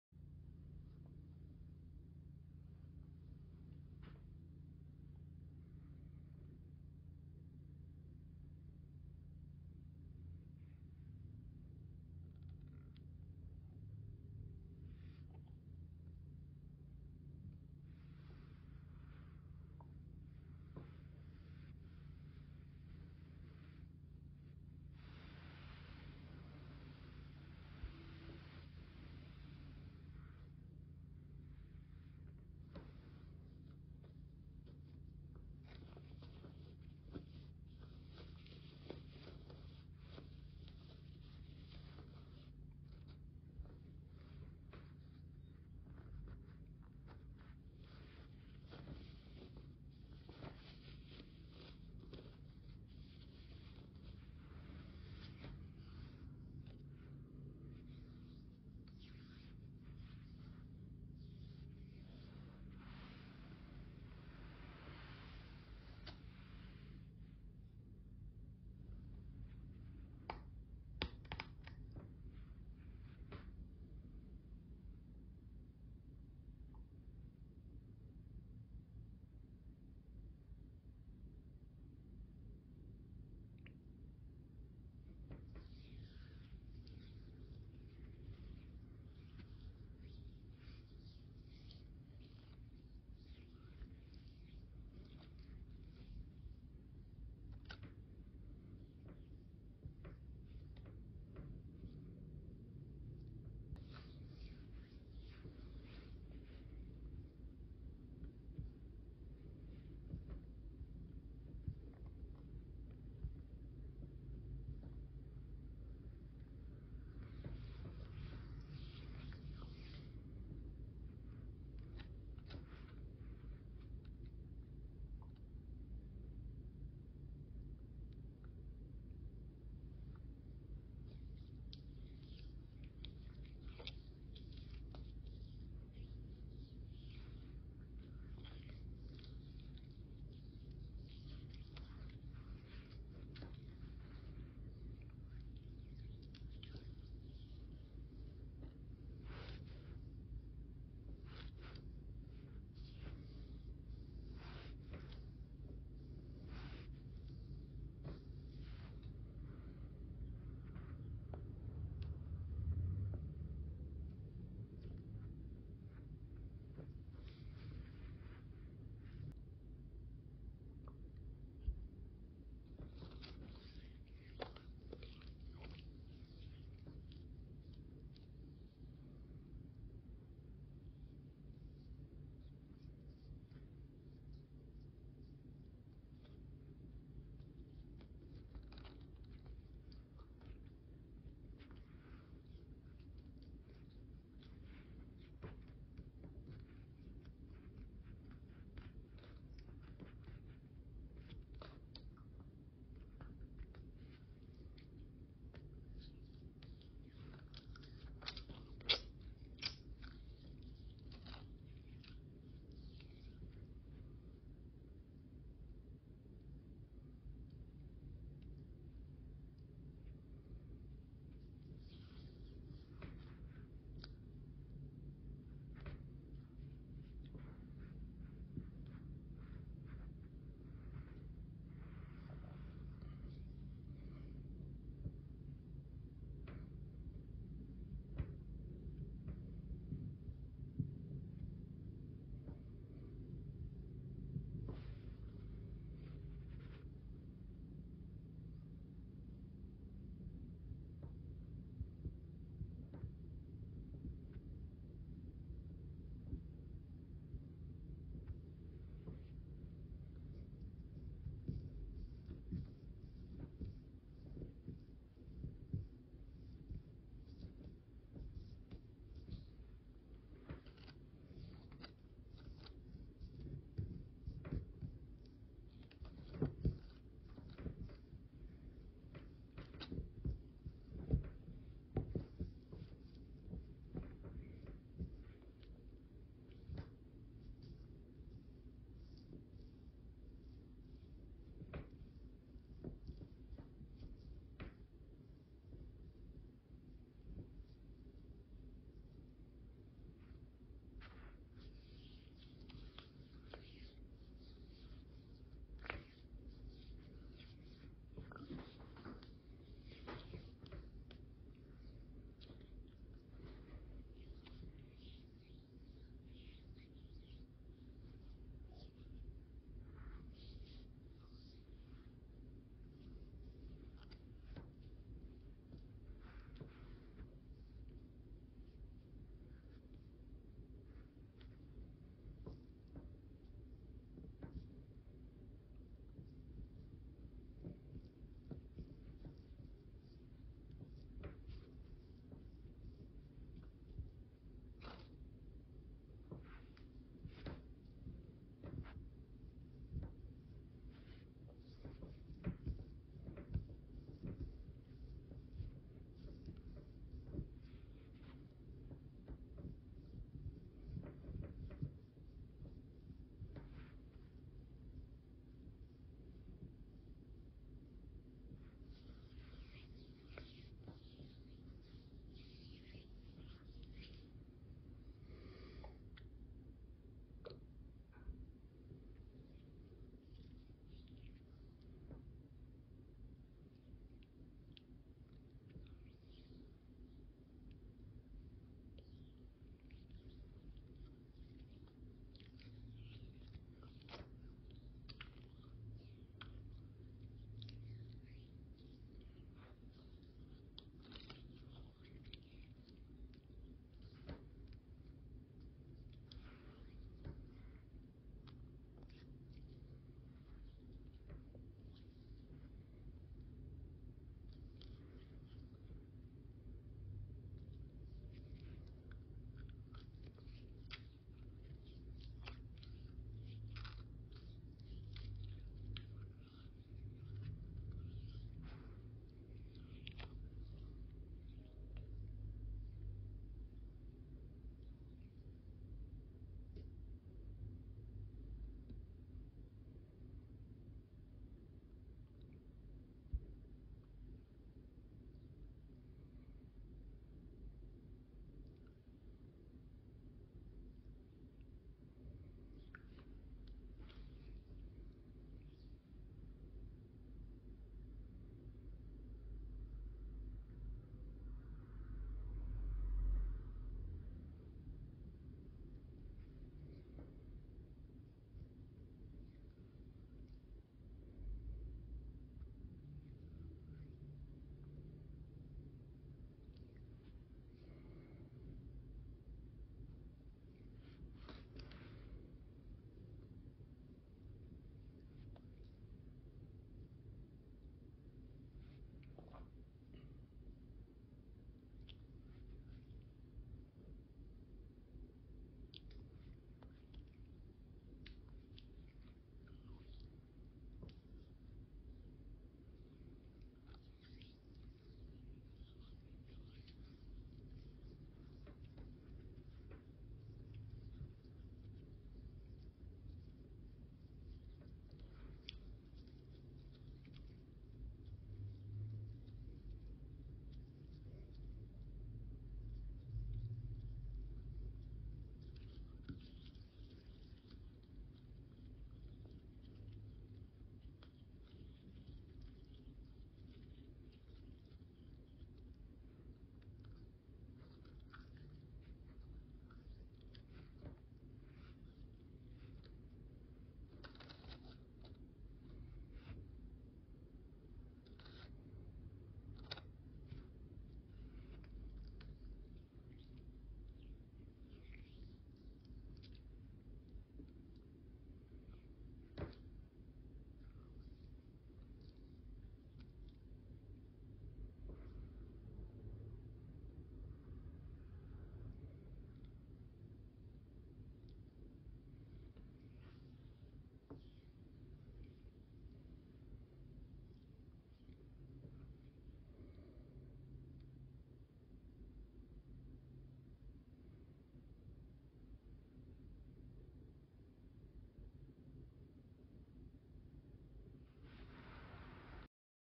ASMR Oily Self-Hand Massage for Sore Phone, Mouse & Keyboard Hands
A guide on giving yourself a hand massage with oil to relieve soreness and fatigue from using your phone, or mouse and keyboard throughout the day.